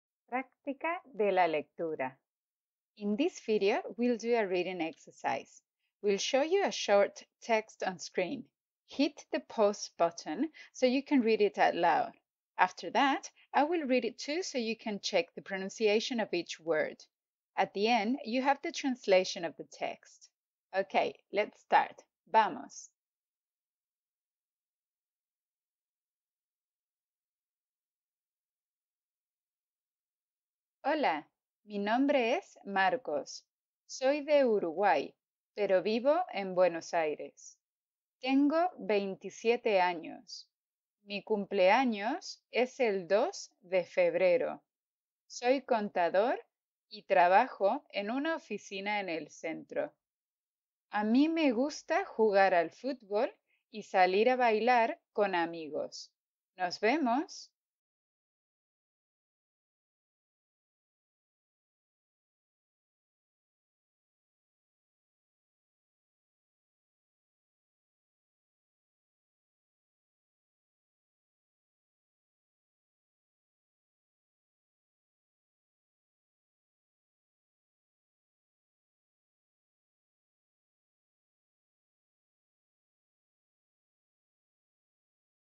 Practice your Reading Skills in Spanish! | HOLA SPANISH
This video is the perfect exercise to practice your reading and pronunciation skills in Spanish. This lesson is ideal for Beginners or A1 students.